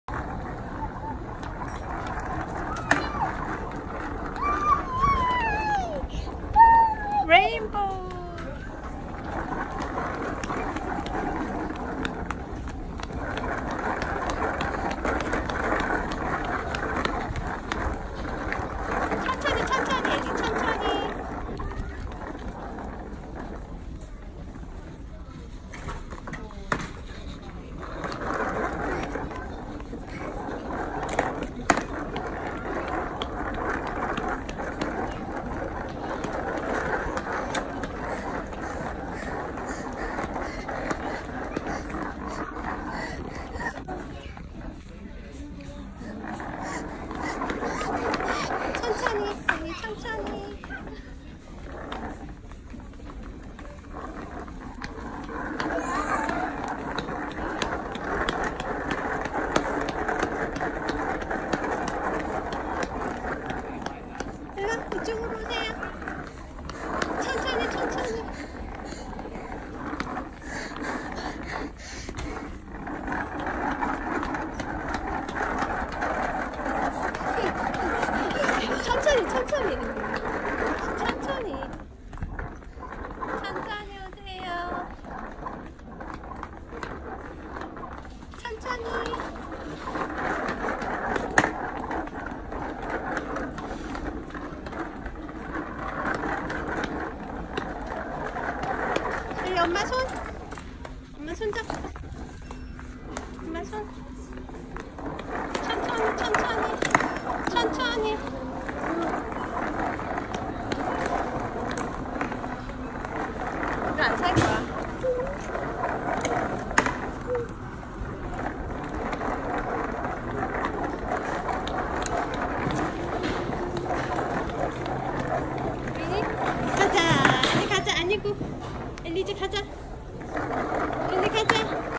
타겟은 놀이터~
타겟에 가면 엘리는 완전 신나!
저 장바구니 너무 사주고 싶었지만
집은 이미 포화상태... 끌고 다닐 수가 없어..
얼마전 에 산 골프섹트도 끌다 말았잖아..ㅋ
타겟 갈 때 마다 가지고 놀면 되지 모 ㅋ

190515